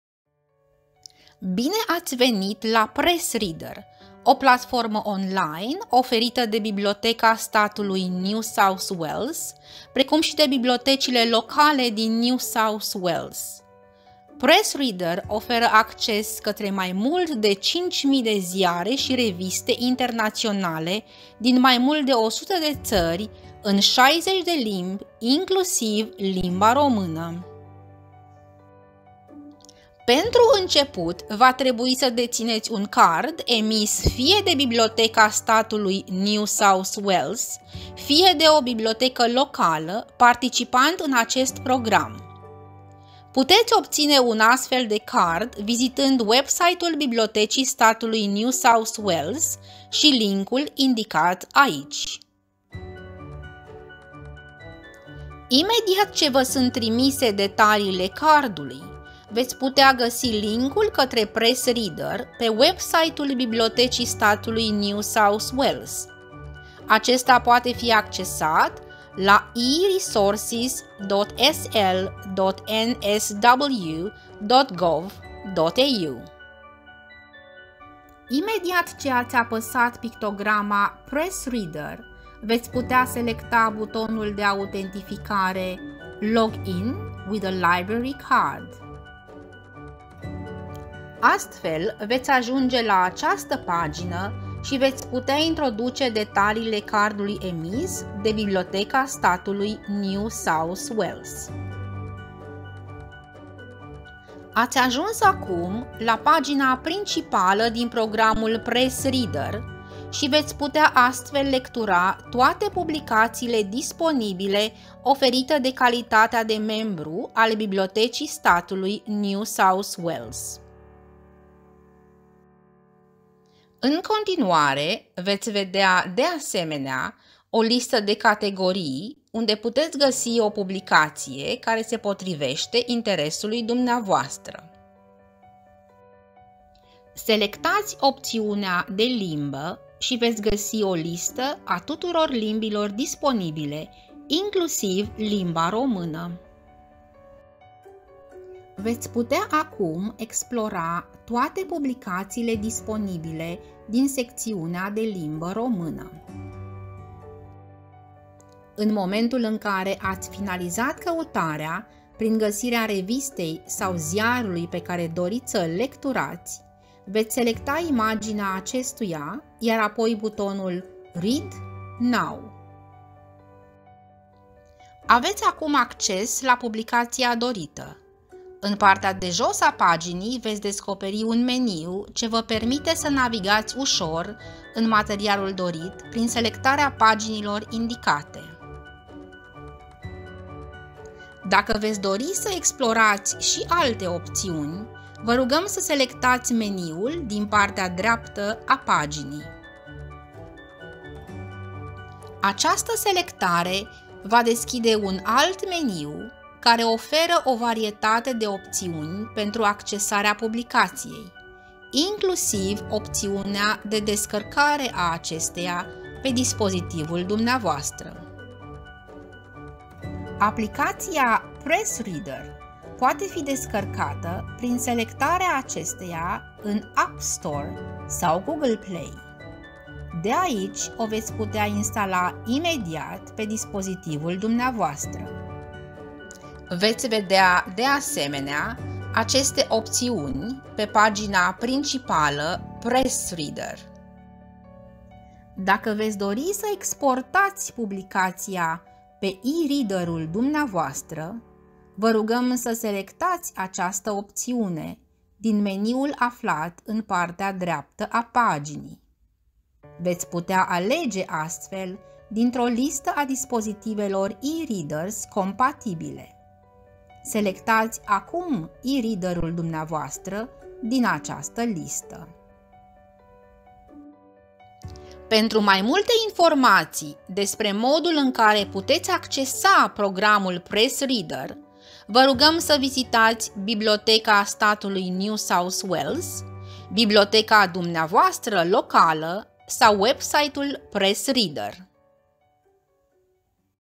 Introducere în Pressreader (Română - Romanian)
Bine ați venit la Pressreader, o platformă online oferită de Biblioteca Națională a statului NSW precum și de Bibliotecile Locale din NSW. PressReader oferă acces către mai mult de 5000 de ziare și reviste internaționale, din mai mult de 100 de țări, în 60 de limbi, inclusiv limba Română.

Vă puteți înscrie pentru un card gratuit oferit de către Biblioteca Națională a statului NSW aici:

Welcome to An Introduction to Pressreader, an online platform provided through the State Library of NSW and Public Libraries in NSW. PressReader offers access to over 5000 international newspapers and magazines from more than 100 countries in 60 languages, including Romanian.